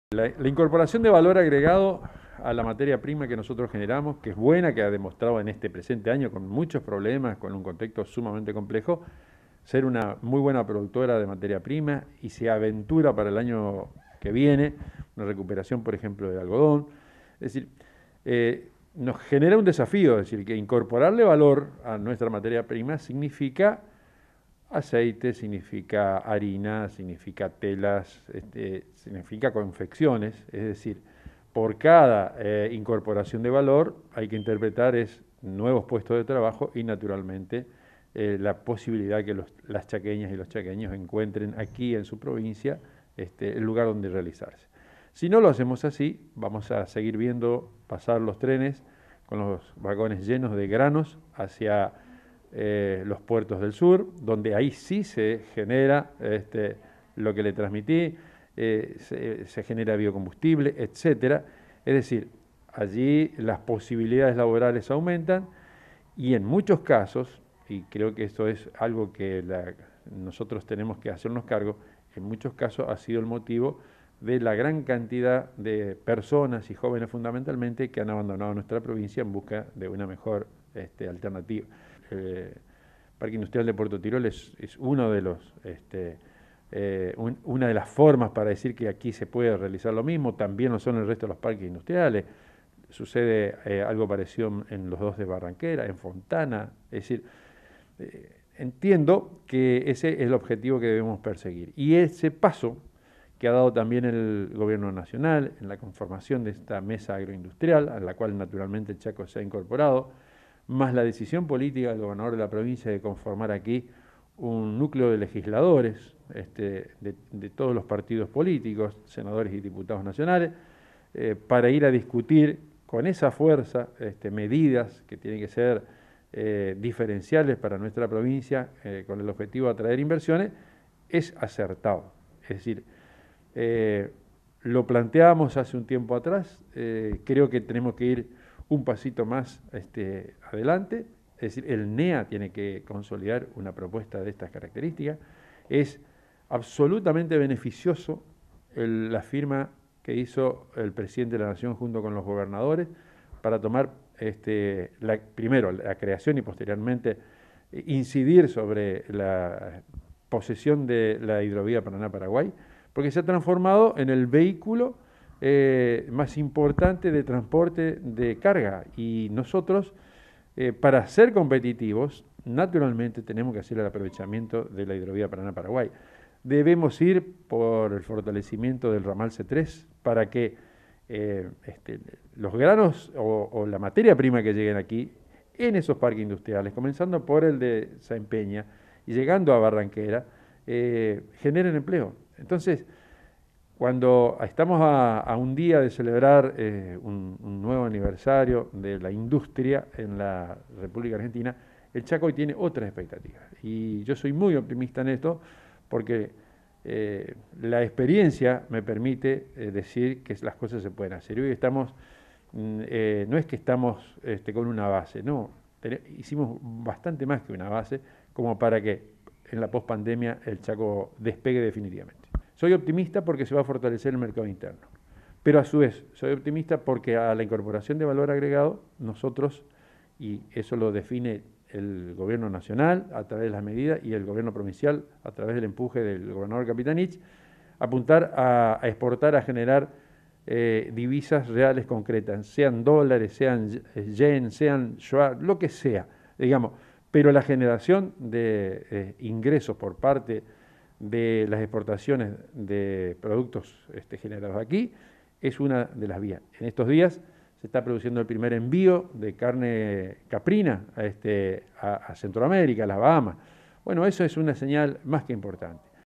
N9 - CHACO: EL CHACO AGROINDUSTRIAL QUE QUEREMOS
PRINCIPALES OBJETIVOS EN UN NUEVO ANIVERSARIO DE LA INDUSTRIA
El presidente de la legislatura chaqueña, Hugo Sager, dialogó con noticiero 9 y se refirió al nuevo aniversario de la industria a celebrarse este martes 2 de septiembre. En este sentido resaltó los avances de la provincia en materia de agroindustrialización, y además los principales desafíos y objetivos de la clase política y empresarial para los próximos años. Se destaca el valor agregado que se le puede dar a nuestros productos primarios, como la generación de mano de obra local y calificada que no requiera de irse a otras provincias para encontrar trabajo digno y de calidad.
HUGO SAGER - PRESIDENTE LEGISLATURA DEL CHACO